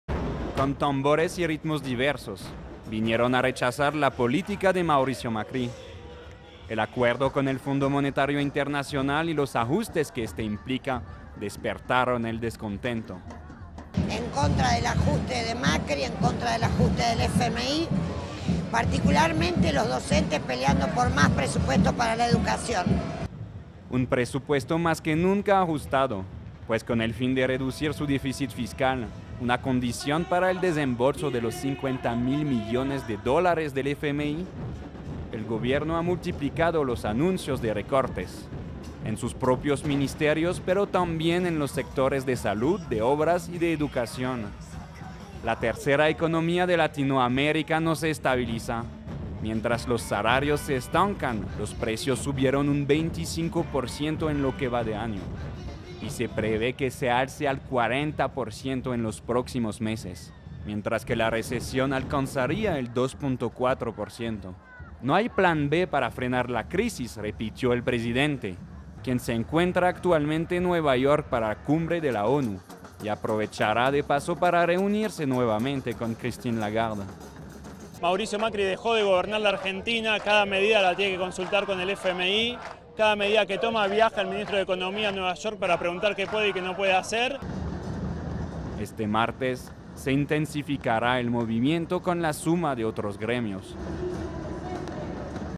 Argentina protestas contra Macri a horas de la huelga general France 24
Los sindicatos y organizaciones convocaron una gran movilización en contra de Mauricio Macri. Rechazan sus políticas económicas y un segundo acuerdo con el FMI, que el mandatario planea cerrar esta semana en Nueva York.